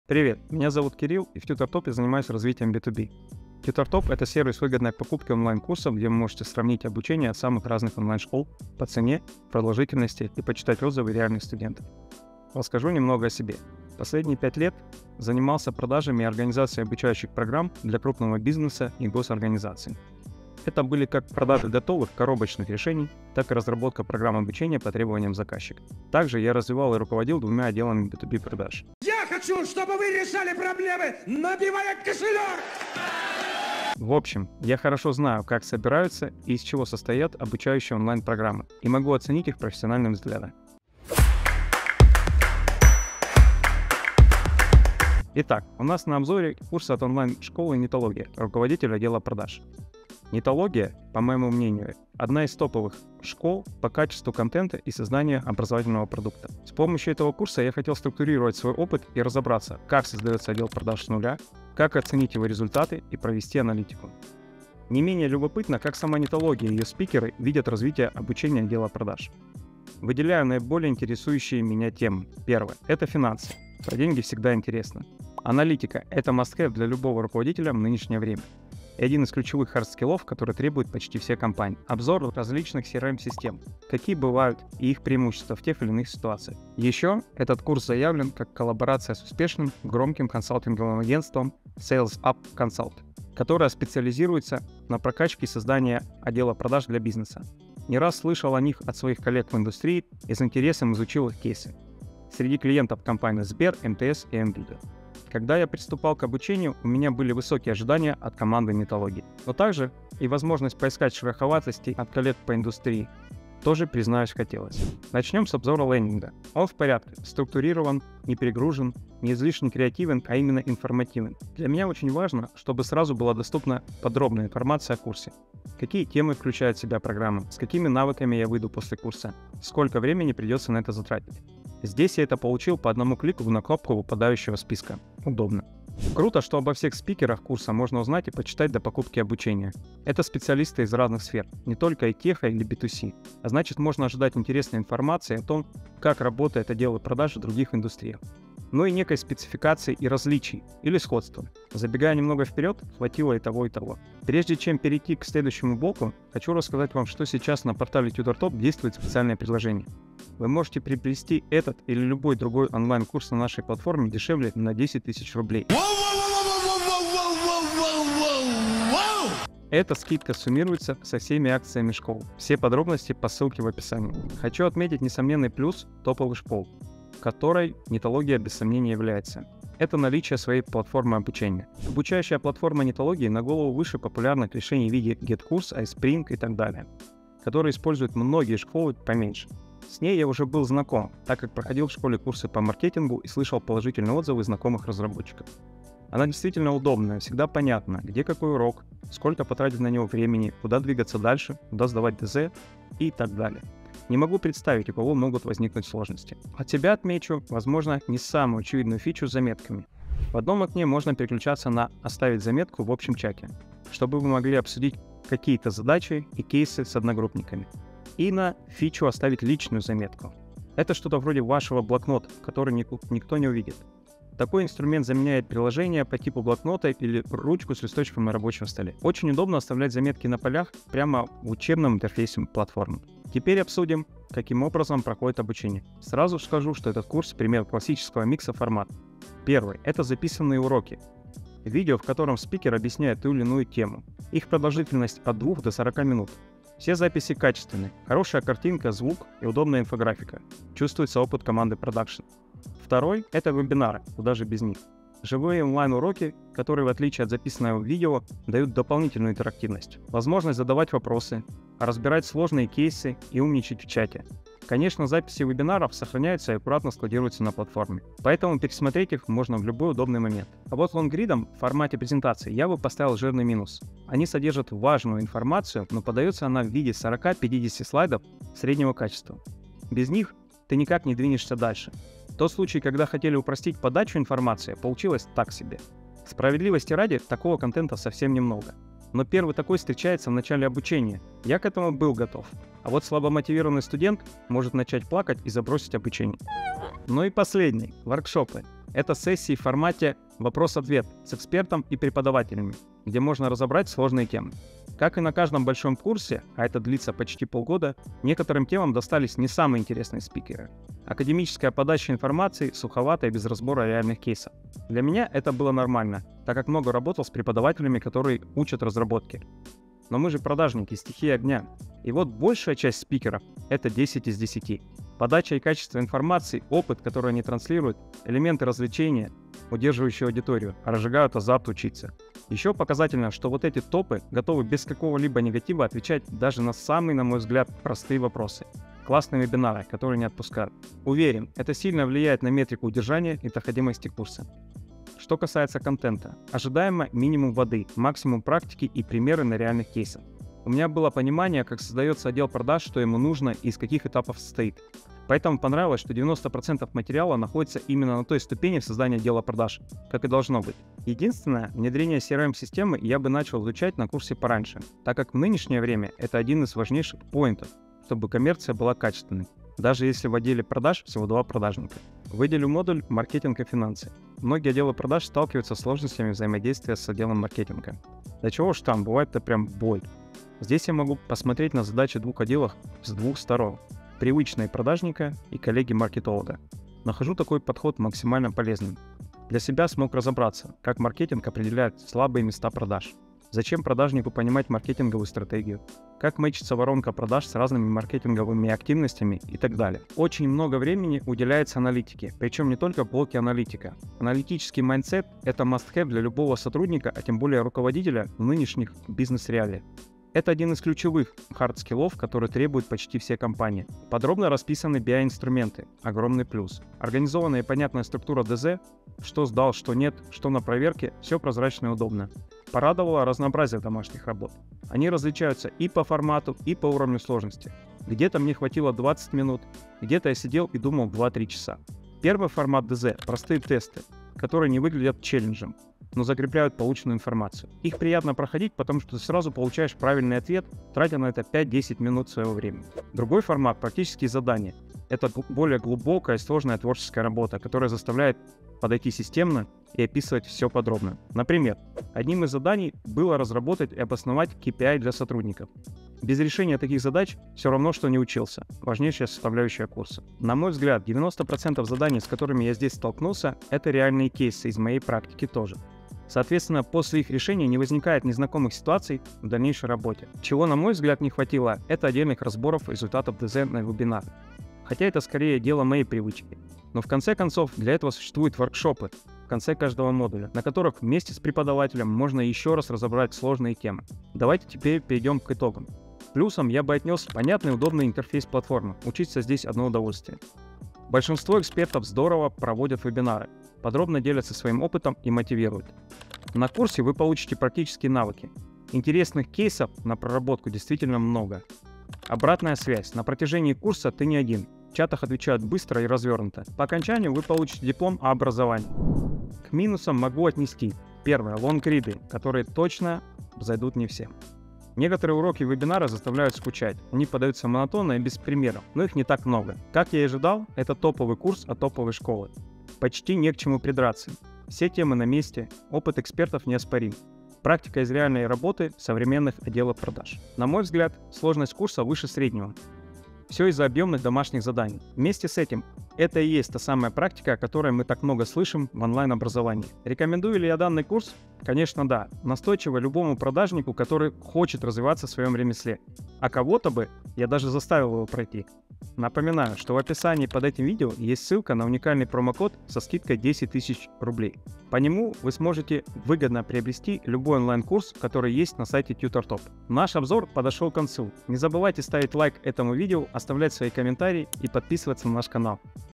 НЕТОЛОГИЯ — ЧЕСТНЫЙ ОТЗЫВ НА ОБУЧЕНИЕ | Разбор онлайн-курса «Руководитель отдела продаж»
В этом видео — обзор онлайн-курса «Руководитель отдела продаж» от Нетологии. Разбираем его содержание, оцениваем достоинства и недостатки. Вы узнаете, какой практический опыт и навыки можно получить после обучения, а мы поделимся своими впечатлениями от его прохождения.

📌 Содержание видео:
0:00 Всем привет!
0:12 О себе. Опыт Кирилла в продажах
00:40 Ожидания от курса «Руководитель отдела продаж»
1:55 Впечатления от лендинга школы
2:16 Кто спикеры курса?
2:34 Дарим 10 000 ₽ на курсы школ-партнёров
2:55 Обзор обучающей платформы
3:53 Как проходит обучение?
5:45 Оценка контента курса
7:02 Домашние задания
8:07 Плюсы курса
8:30 Минусы курса
8:40 Итоговая оценка
9:09 Дарим 10 000 ₽ на курсы школ-партнёров
9:20 Спасибо за просмотр!